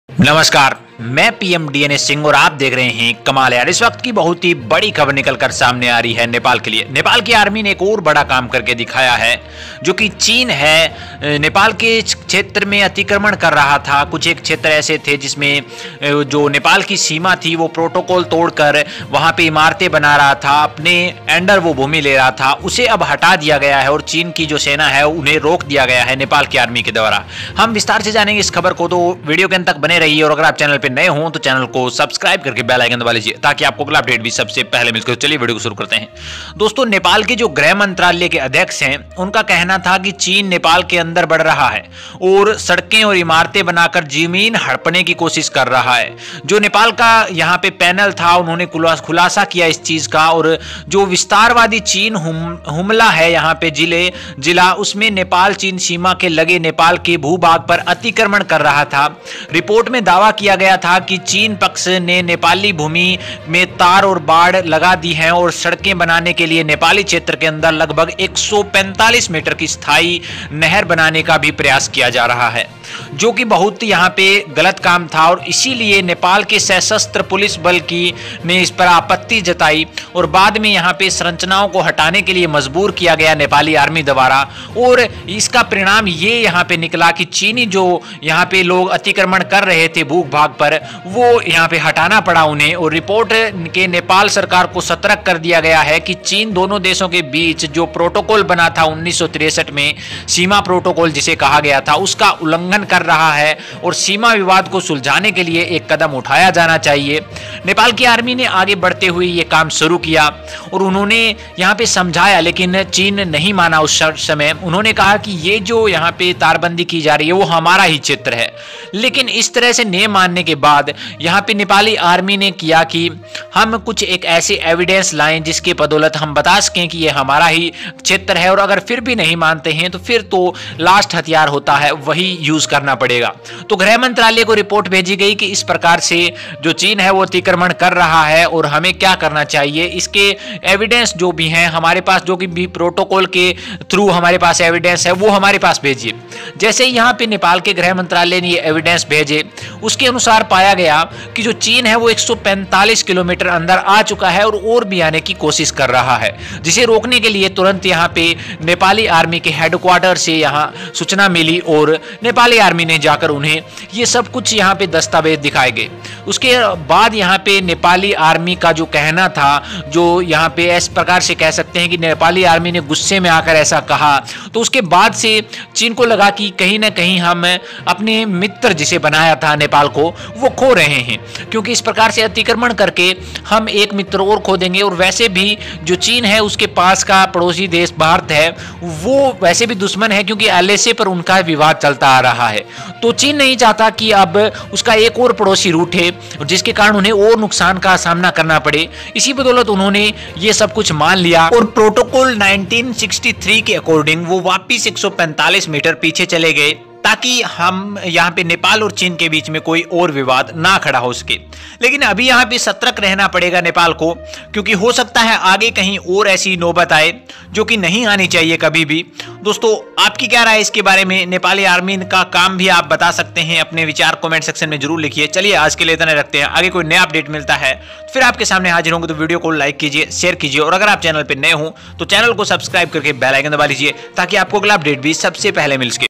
नेपाली आर्मी ने दिया चीन को बड़ा झटका ! Nepali army on Nepal china border ! Nepali army news ! Nepal
Study panel of Nepal home ministry says that China is trying to grab land by building fences, roads and buildings inside Nepal
A study panel of the Ministry of Home Affairs of Nepal has revealed that expansionist China is encroaching upon the territory of Nepal along the Nepal-China border in Humla district. The report claimed that the Chinese side put up wires and fencing in the Nepali land and also tried constructing a permanent canal 145 meters inside the Nepali territory to build a road. The structures were destroyed following the objection of Nepal’s Armed Police Force. The report has alerted the Nepal government that China is violating the 1963 Boundary Protocol between the two nations and steps to be taken to resolve the border dispute.

The report, submitted to Home Minister Bal Krishna Khand recently, identifies several problems near the Nepal-China border in Humla. In August, the Ministry of Home Affairs of Nepal had formed a six-member panel headed by Joint Secretary Jay Narayan Acharya to probe and report the land encroachment in Humla
There are as many as 10 border posts separating the border between Nepal and China in the Humla district and not a single post left which China has not either altered or tried to do. The panel is stated to have visited boundary pillars namely 5, 6, 7, 8, 9, 10, 12 and gathered reports of other posts. The panel found the Chinese side having fenced and wired pillar 6 (1) which falls in the Nepali territory. It has been done to show its presence in the areas between pillar 6 (1) and pillar 5 (2). Again the pillar 7(2), on the Chinese side was missing. The Chinese side has erected fences towards pillar 10, which is 32 meters from Nepal-China common pillar 9(2). Nepali citizens are prevented from grazing their cattle in the areas between pillar 5 (2) and pillar 4.

The panel also visited Lapcha Bhanjyang with Pillar No. 12 where China has built as many as nine buildings in the village of Nepal. A local village council Vishnu Bahadur Lama had alarmed the government of Nepal about China grabbing a village in the country.

However, people and politicians of Nepal have been warning that China has a dangerous agenda to occupy the territory of Nepal. Last year Nepali Congress MP from Karnali Province, Jeevan Bahadur Shahi had led a team to investigate the encroachment of Nepal’s land in Humla. But the then Nepal Prime Minister Khadga Prasad Sharma Oli of the Nepal Communist Party was under influence of China. He remained busy in the anti-India campaign along with China instead of protecting the sovereignty of his nation. In his report, Jeevan Bahadur Shahi had accused China of encroaching on the territory of Nepal in Huml